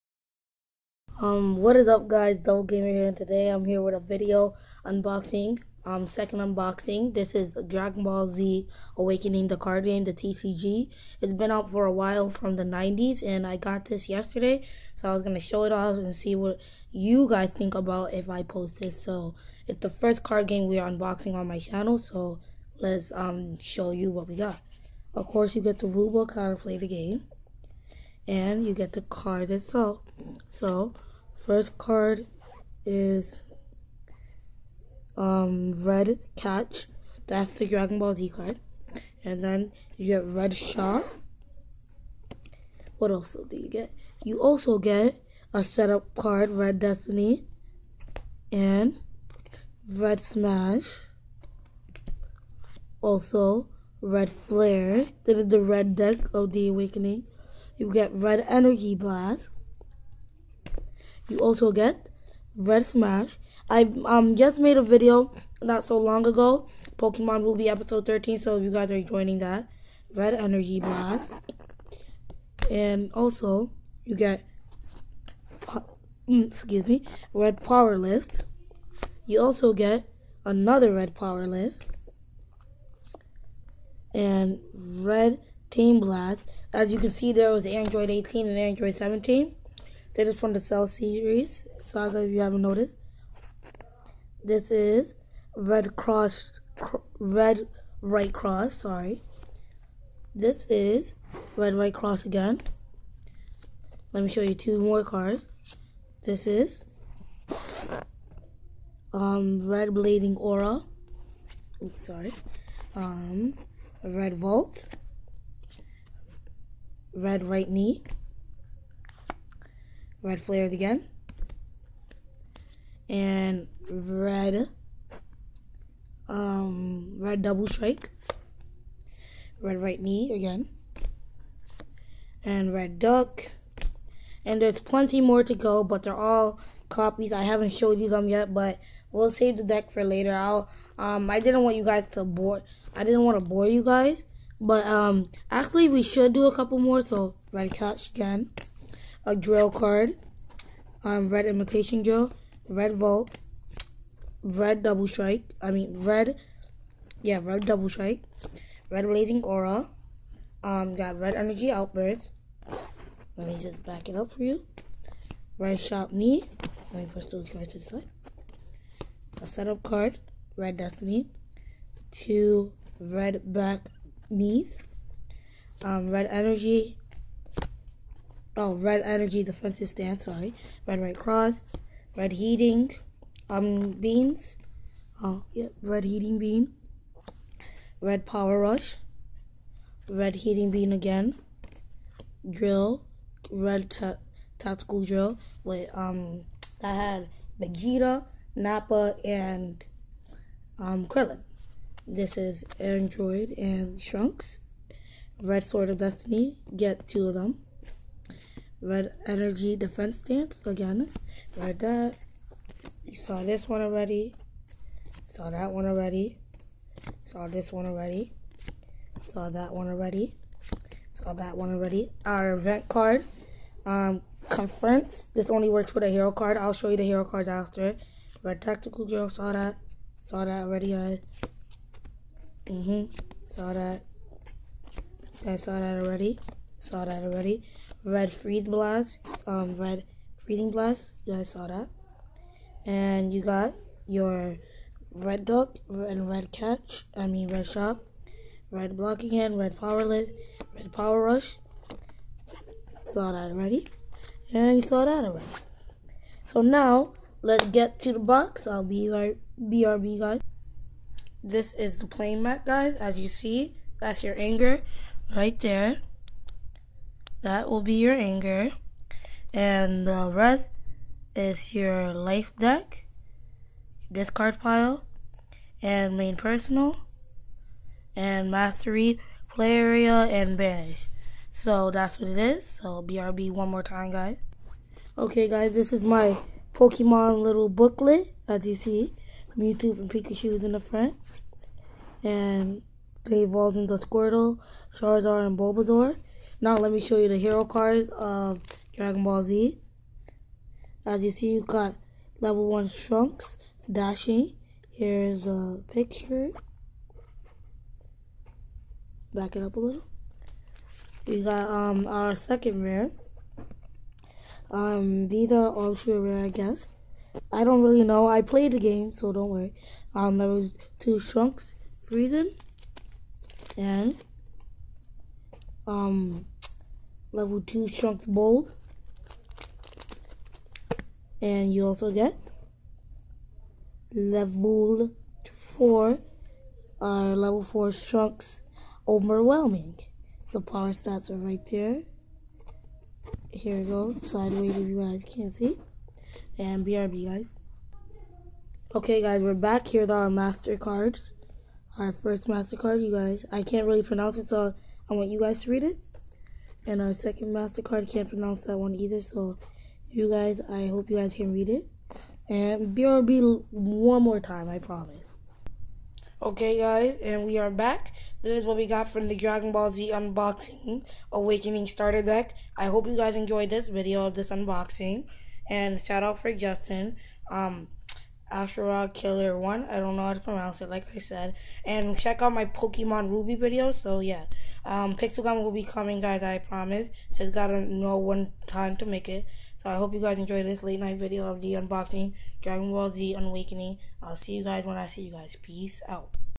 DBZ Awakening Tcg#part 1
Shoutout for Justin# II unboxing on my channel and this time is DBZ Awakening Trading Card Game so I hope you guys enjoy this video and pixel gun will be coming I promise he's got to find a time when to make it☺😎😀